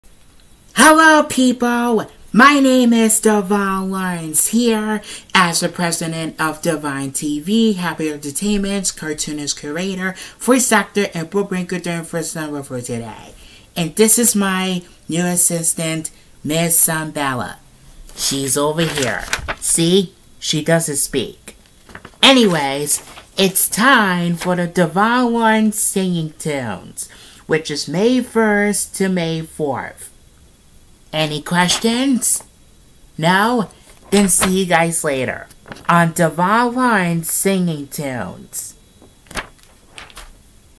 DEVON LAWRENCE SINGING TUNES is available now!
Hi folks!  My name is Devon Lawrence & this is my new assisant Miss Sun Bella,  I can sing 7 songs tomorrow.  Hope you're enjoy this video.